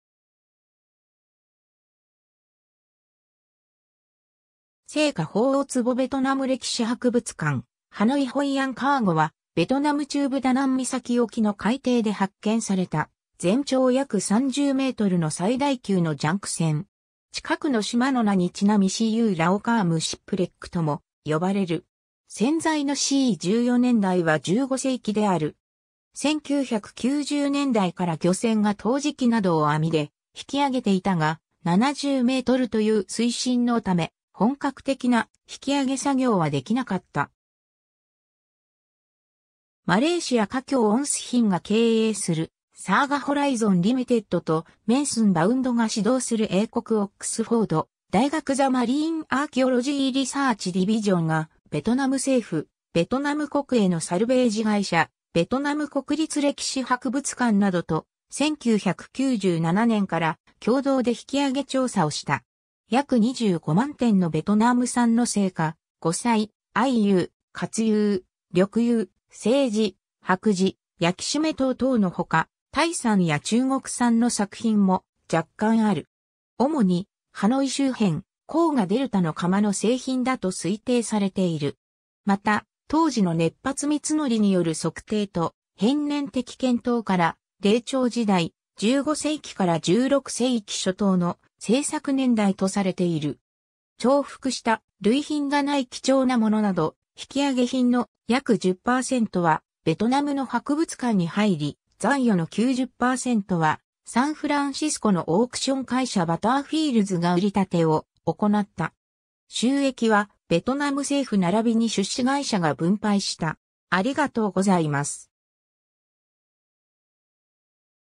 ホイアン・カーゴ
青花鳳凰壺　ベトナム歴史博物館、ハノイ ホイアン・カーゴ（Hoi An Cargo / Hoi An Wreck）は、ベトナム中部ダナン岬沖の海底で発見された全長約30mの最大級のジャンク船。
近くの島の名にちなみCu Lao Cham shipwreckとも呼ばれる。
船材（チーク材）のC14年代は15世紀である。
1990年代から漁船が陶磁器などを網で引き揚げていたが、70mという水深のため、本格的な引き揚げ作業はできなかった。
 マレーシア華僑On Soo Hinが経営するSaga Horizon Limitedと、Mensun Boundが指導する英国オックスフォード大学the Marine Archaeology Research Divisionがベトナム政府、ベトナム国営のサルベージ会社､ベトナム国立歴史博物館などと､1997年から共同で引揚げ調査をした｡ 約25万点のベトナム産の青花･五彩･藍釉･褐釉･緑釉･青磁･白磁･焼締陶等のほか、タイ産や中国産の作品も若干ある。
主にハノイ周辺、紅河デルタの窯の製品だと推定されている。
また、陶磁の熱発光法（フレミング法）による測定と編年的検討から、黎朝時代・15世紀～16世紀初頭の製作年代とされている。
 重複した類品がない貴重なものなど引き上げ品の約10％はベトナムの博物館に入り、残余の90％はサンフランシスコのオークション会社Butterfieldsが売り立てを行った。
収益はベトナム政府ならびに出資会社が分配した。